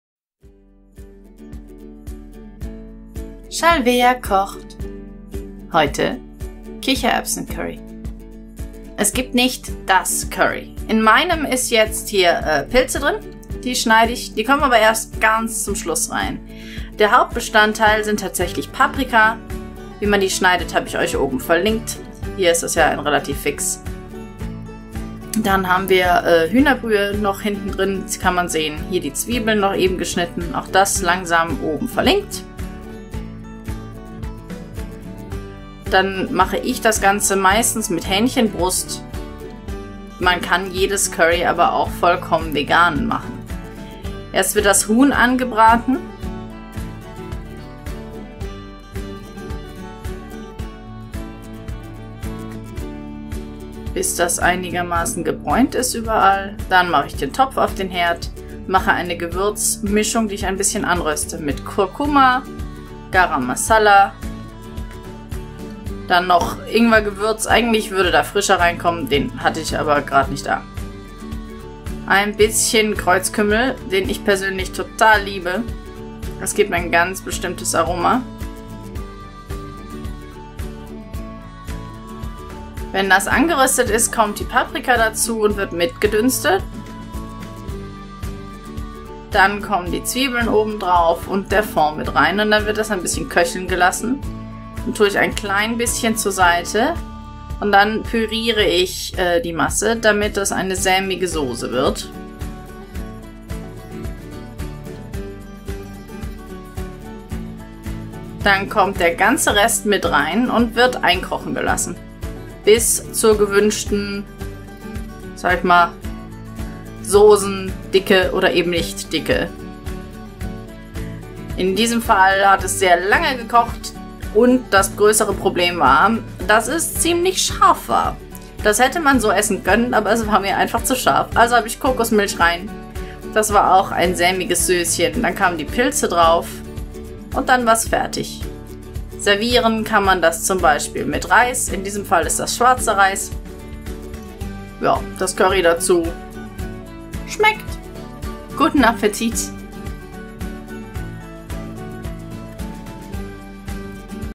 Shalvea Kocht - Kichererbsen Curry
Über Curry, was es ist und was es nicht ist, wurde in der Geschichte der Küche schon viel debatiert.
Generell zu sagen ist, dass es nicht "das Curry" gibt.
In etwa so, wie es ja auch nicht "Das Brot" gibt, das einzige seiner art und nichts anderes heißt so.

ich mache sehr viele unterschiedliche variationen, das hier ist aber die häufigste

Zutaten:

Hächnchenbrust
Paprika
Champignons
Zwiebeln
Geflügel Fond
Gewürze : Kreuzkümmel, Kurkuma, Garam Massala, Cayenne Pfeffer

und wems zu scharf oder nicht cremig genug ist:
Kokosmilch